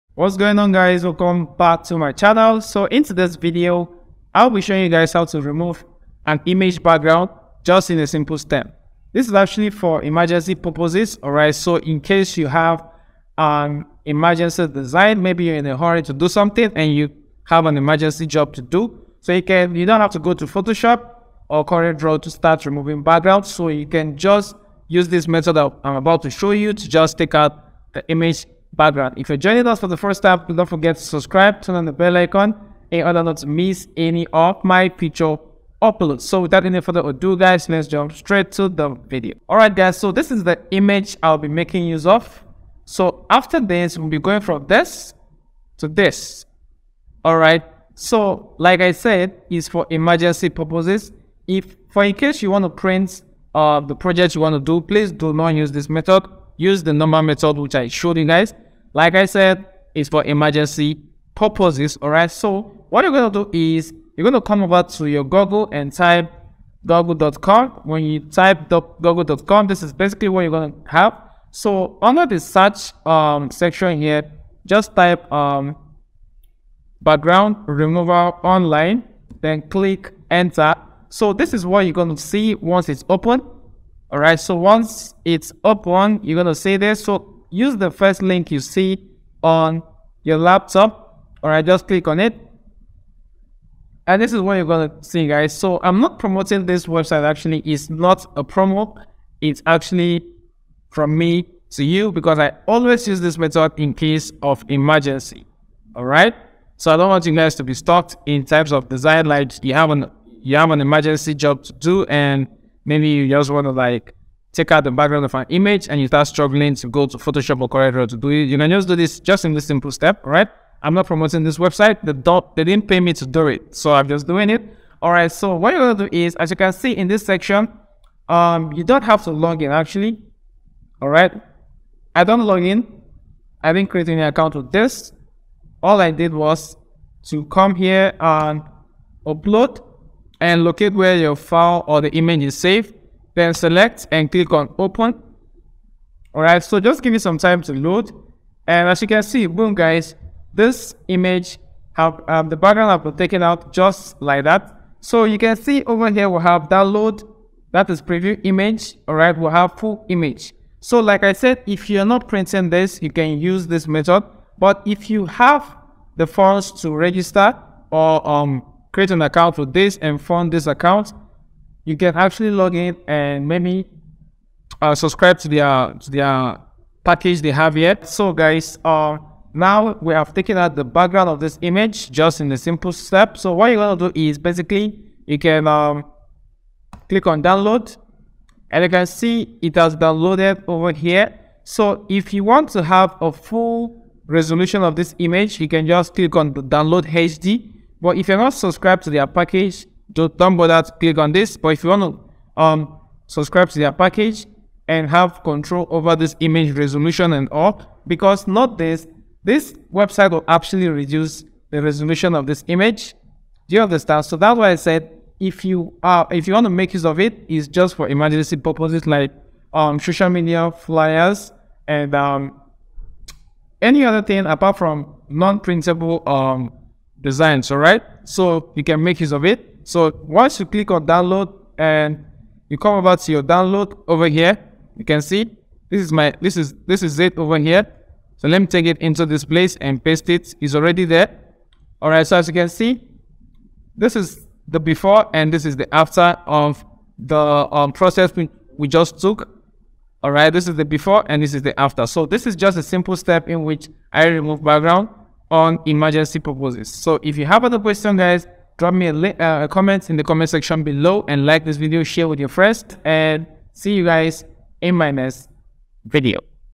How to Remove Background Fast (For Beginners)
In this video i will show you, how to remove image background in a simple step.

H  O  W   T O        R  E  M  O  V  E      B  A  C  K  G  R  O  U  N  D

D O N T   M  I  S  S    O  U  T